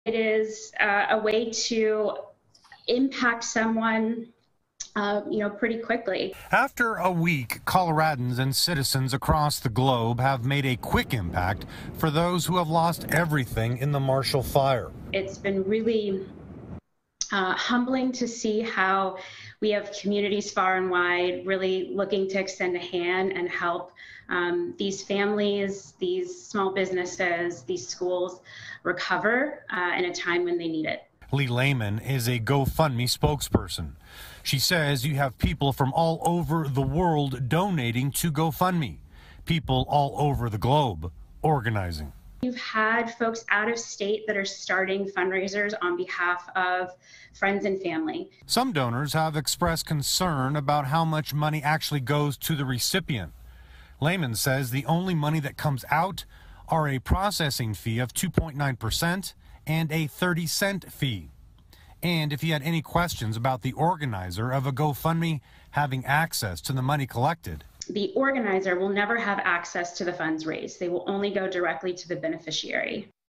GoFundMe pages raise $18 million for Marshall Fire families
A GoFundMe spokesperson says more than 1,300 fundraising efforts have been started to help Marshall Fire Families, raising $18 million. Greg Nieto reports.